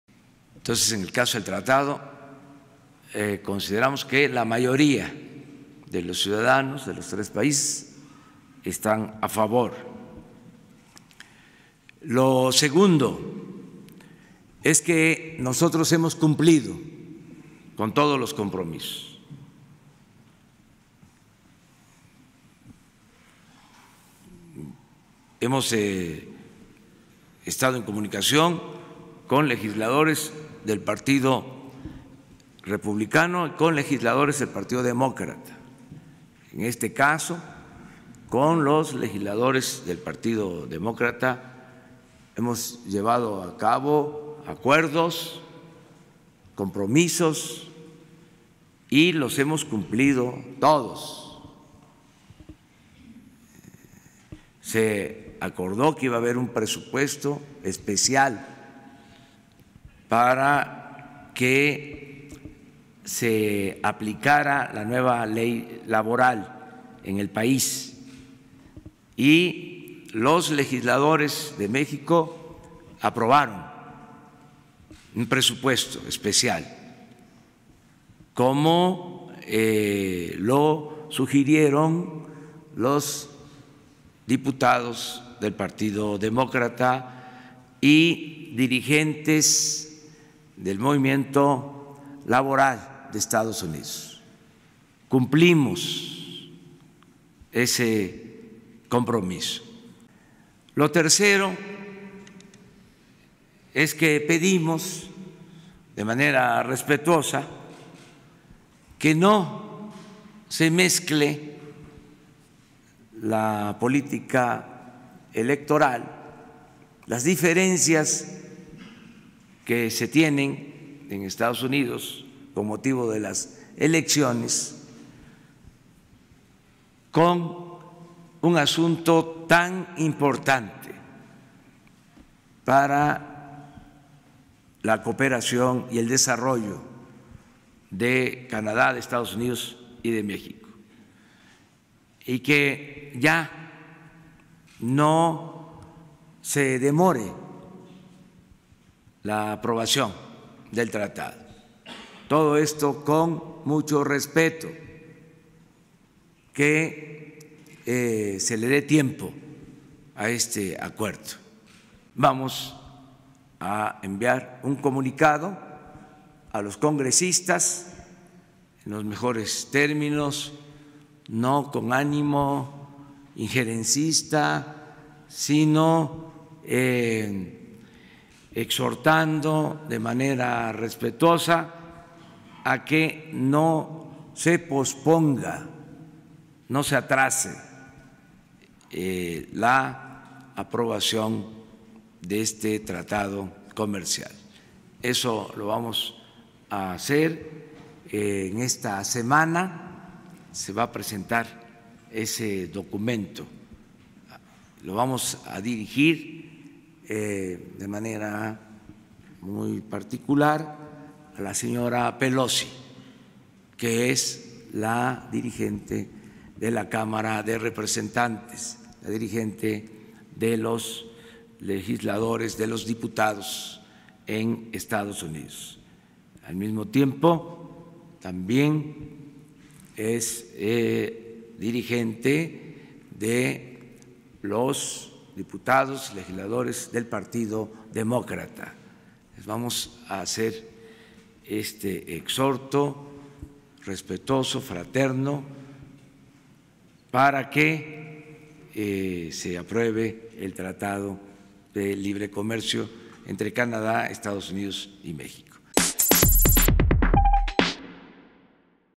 Pedimos que no se mezcle la política electoral con un asunto tan importante como el TMEC: AMLO
El presidente Andrés Manuel López Obrador dijo que su gobierno ha cumplido todos sus compromisos en materia del TMEC y recalcó que espera que no se retrase más su aprobación, por lo que pidió no se mezcle con la política electoral de EEUU